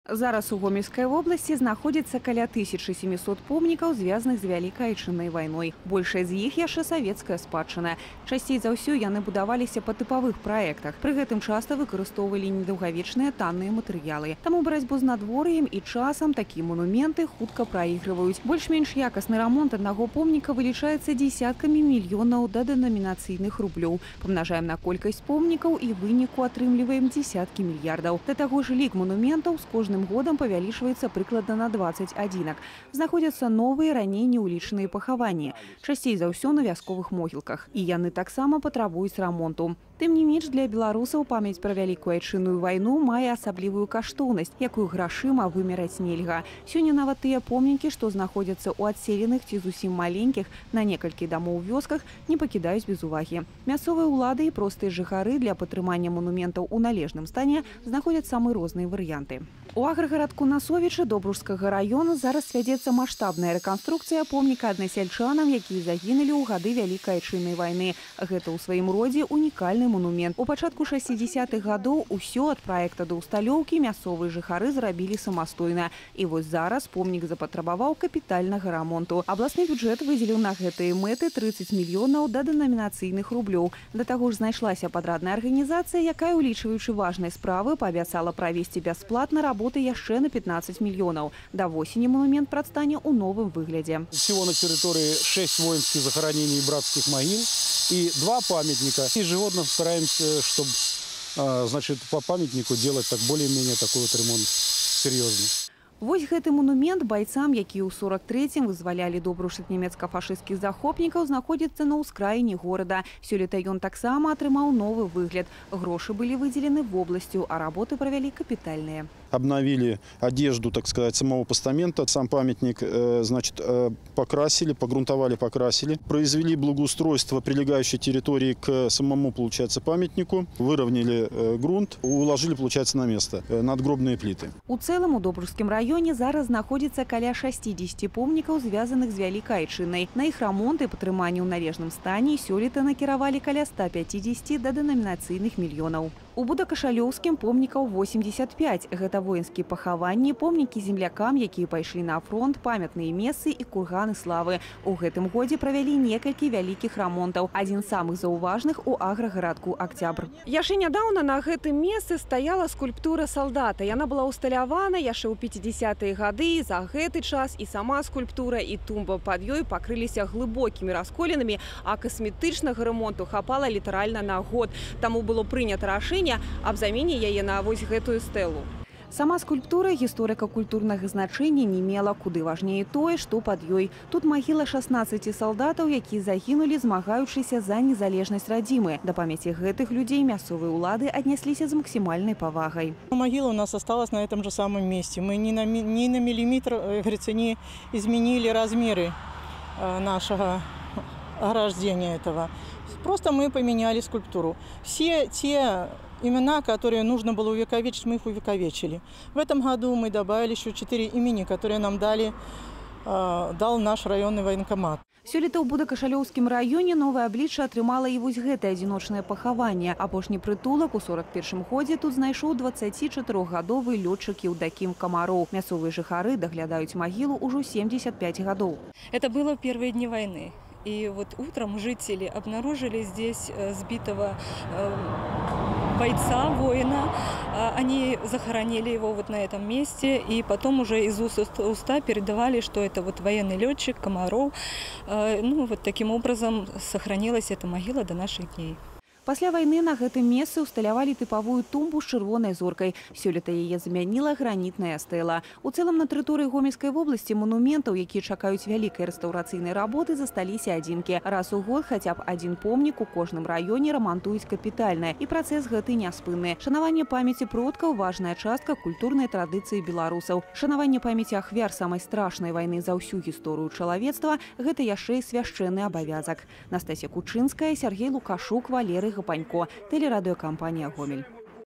Благоустройство памятников Великой Отечественной войны
Около трех миллиардов неденоминированных белорусских рублей в этом году были выделены на содержание памятников Великой Отечественной войны в Гомельской области. Как осваиваются эти деньги? В каком состоянии находятся братские могилы и памятные знаки землякам, погибшим во время Великой Отечественной? Наш корреспондент Анастасия Кучинская заинтересовалась этой темой.